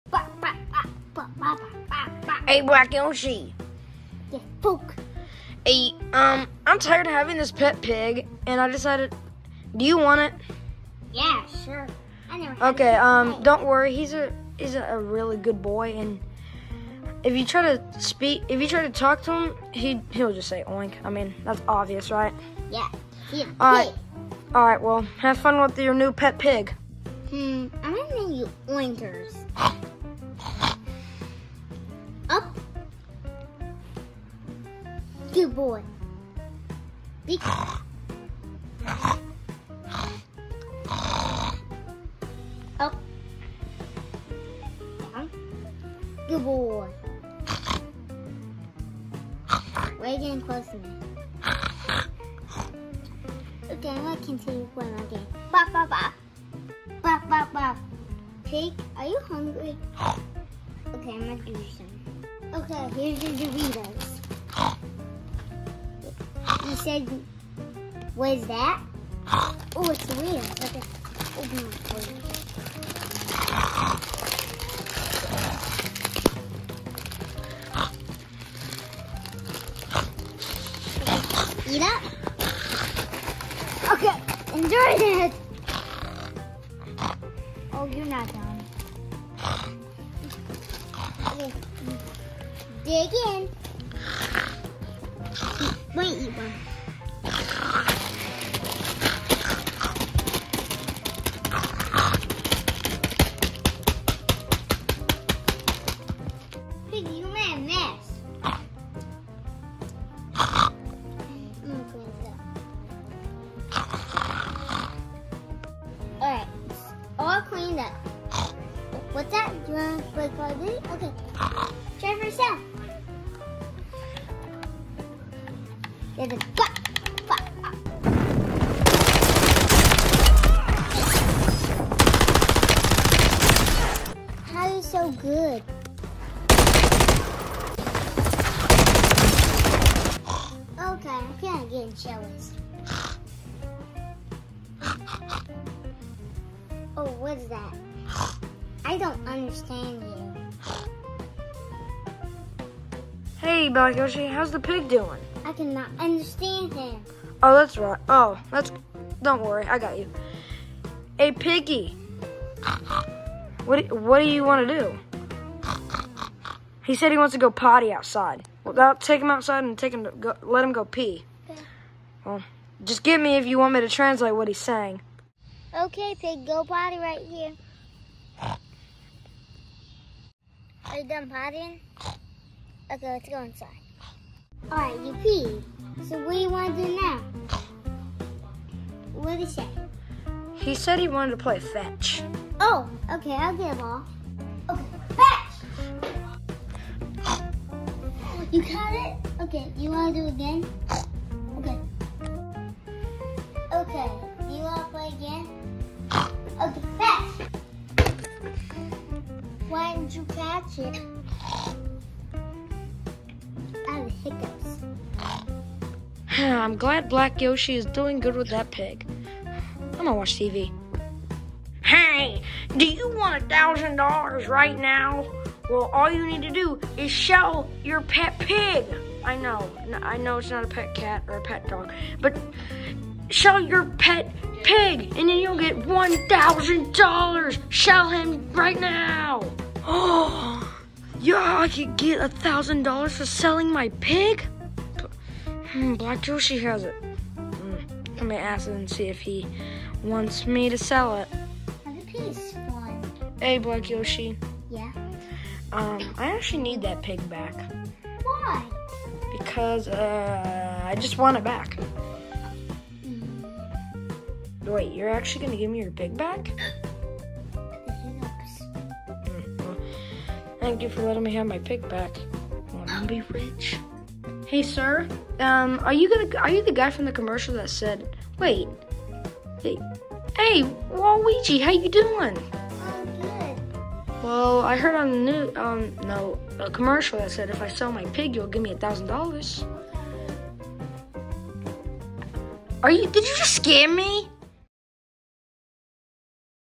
SDJ Video: Black Yoshi’s Pet Pig! 🐖
Black Yoshi gets a pet big from Wario!